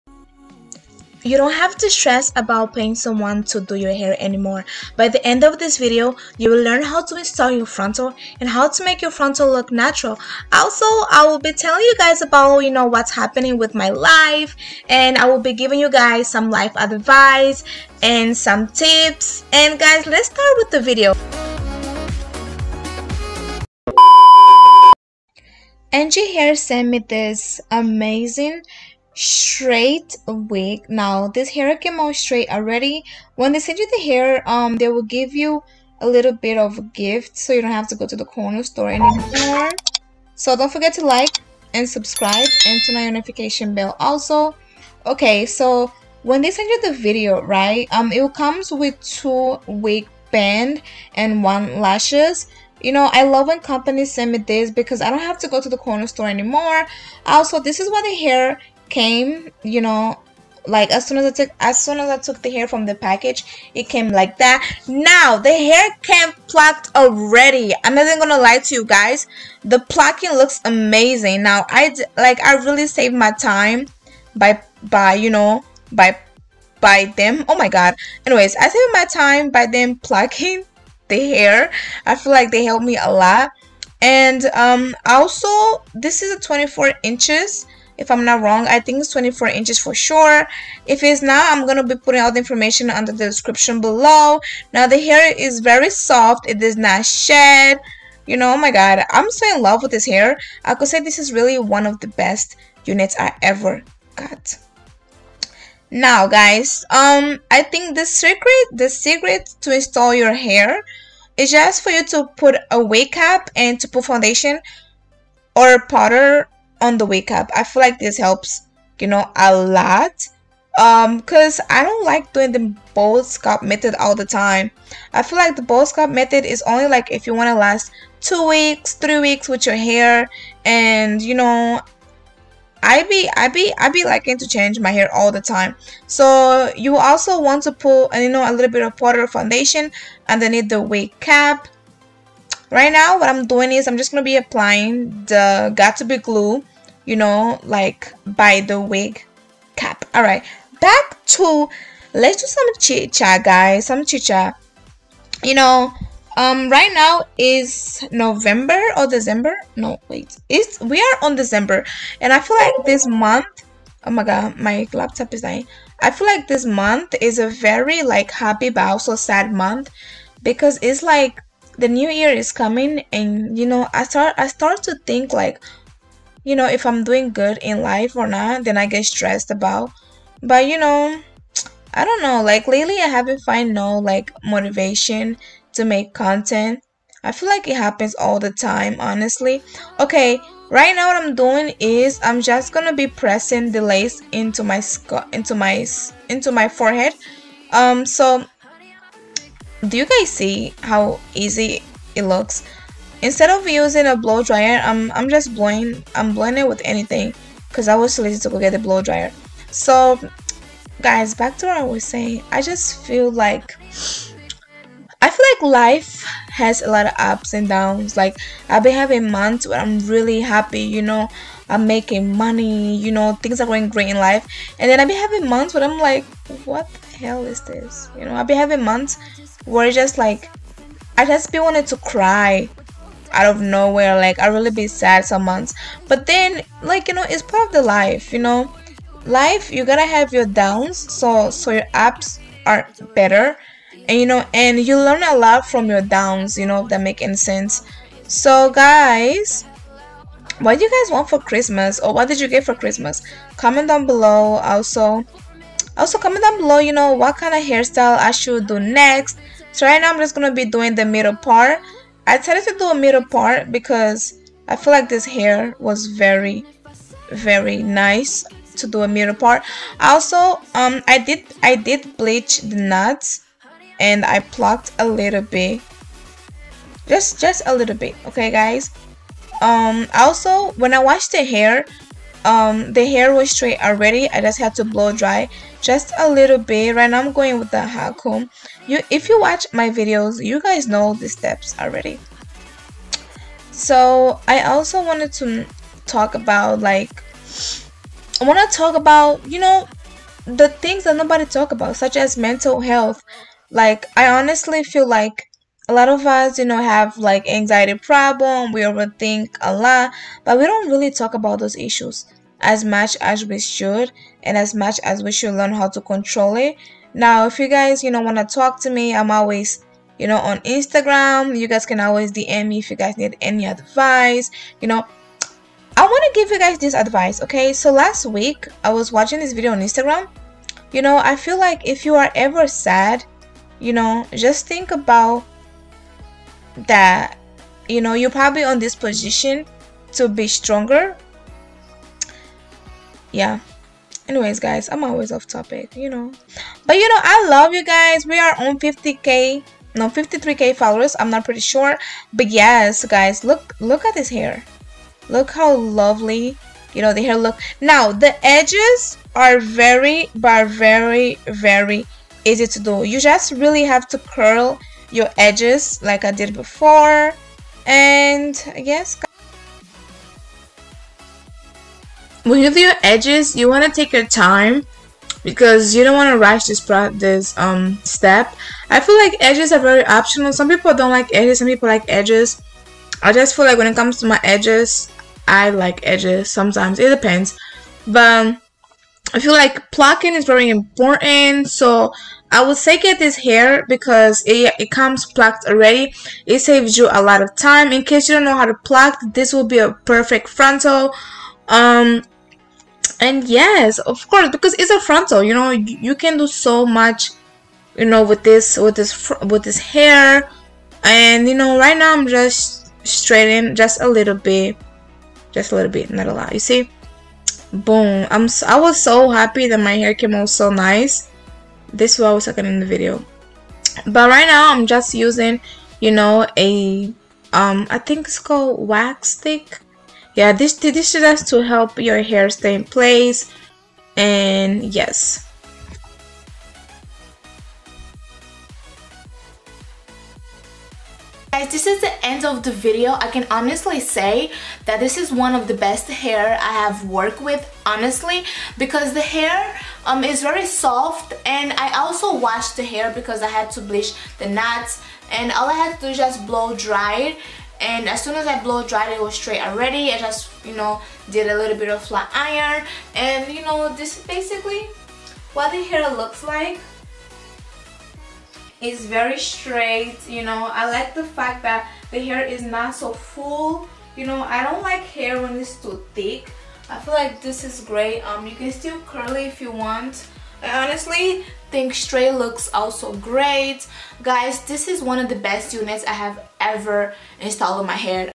CHIT CHAT GRWM LIFE UPDATE + THE EASY WAY TO INSTALL YOUR FRONTAL FT ANGIE QUEEN HAIR
●▬▬▬▬▬ Angie Queen Hair ▬▬▬▬▬●
Hair Details: AngieQueen Transparent /HD Brazilian Silky Straight Natural Color 13X4 Long Lace Front Human Wig : 24 inches
Merry Christmas